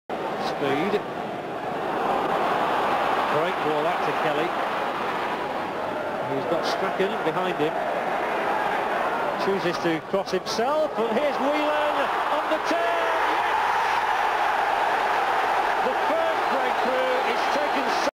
Noel Whelen scores for Leeds United vs Sunderland 6/10/93
This is Noel Whelan scoring for Leeds United in a 2:1 defeat at home to Sunderland on 6th October 1993 in a League Cup game.